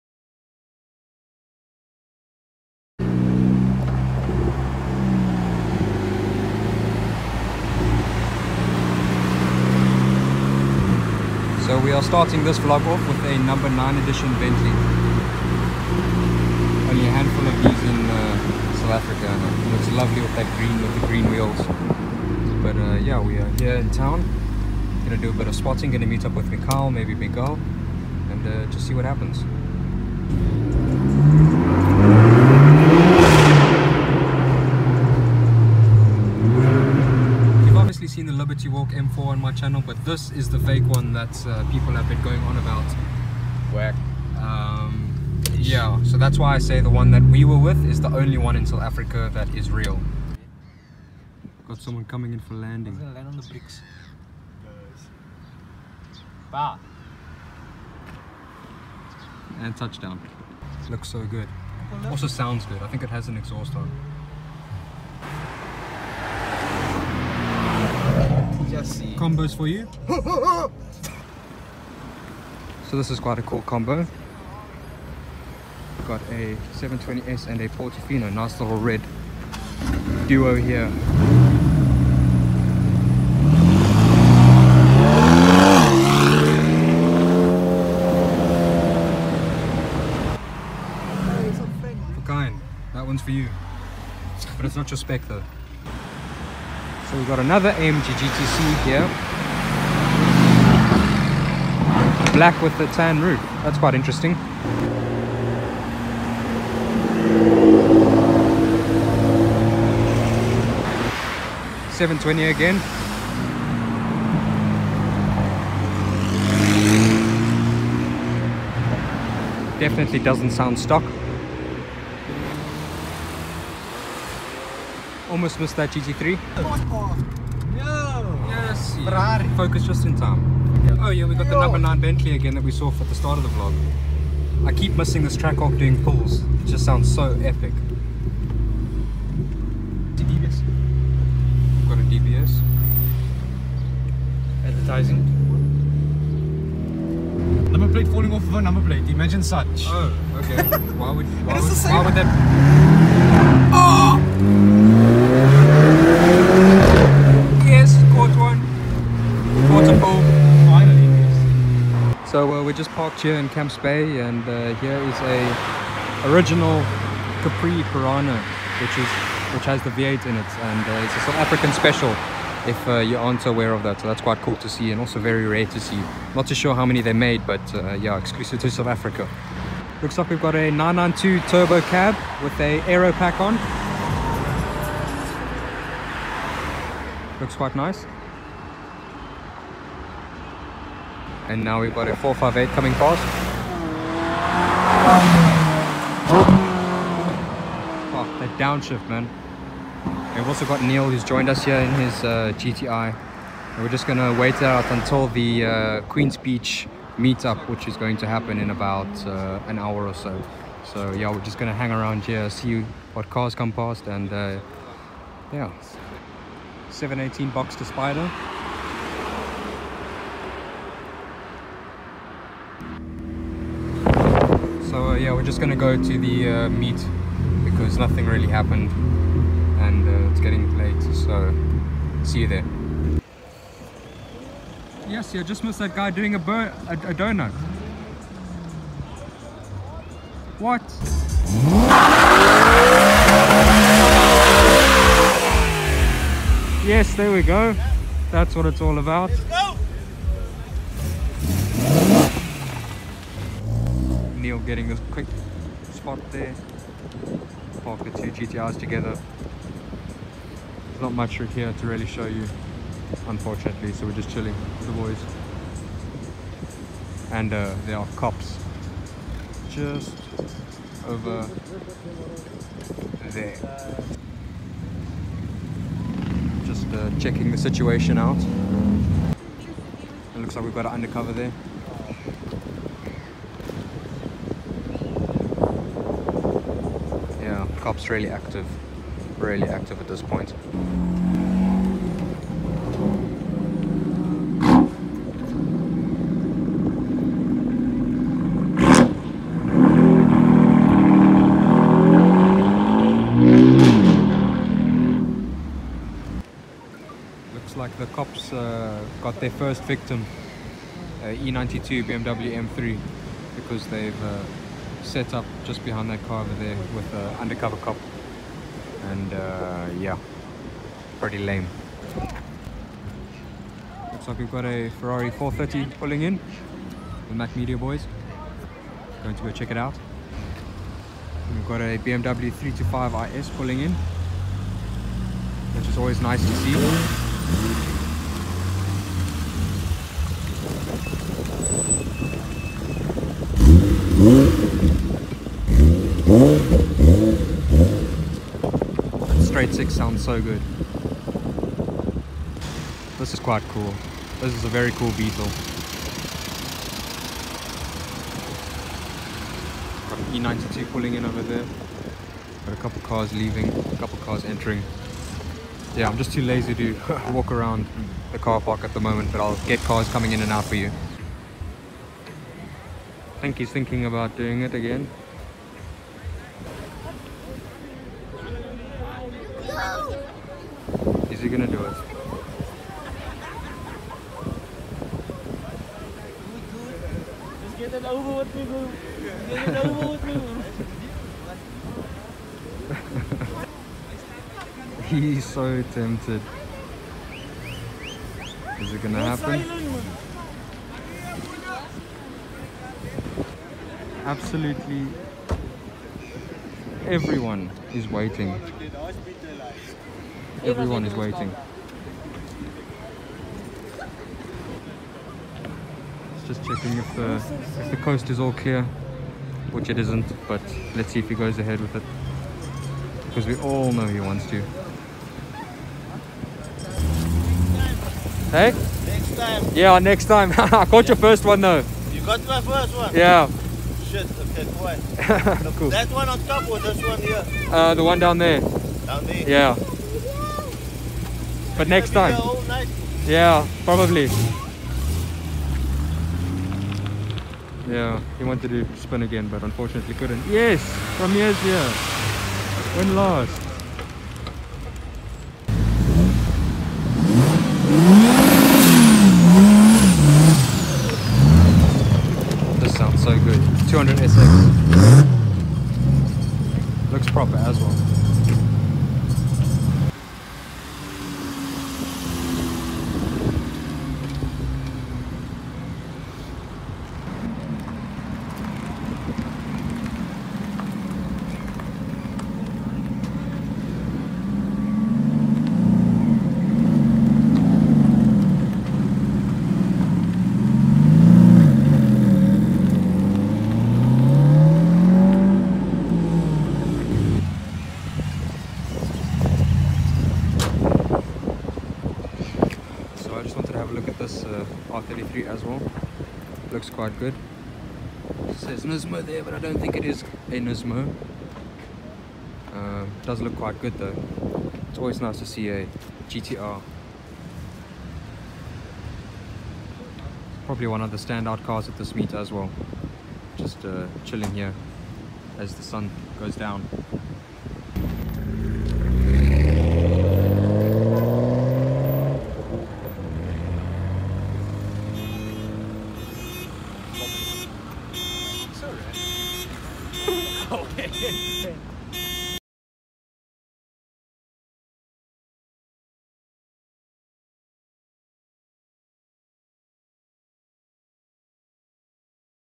Car Spotting Until The Queens Beach Park-Off | JDM, BMW M, Supercars
00:00 - Intro
00:03 - Entering Town
00:38 - Car Spotting in Sea Point
02:07 - Heading to Camps Bay
03:44 - Heading to the Park-Off
06:12 - BMW M3 Restricted by the Cops
07:37 - Cars Start Leaving
08:23 - R33 GTR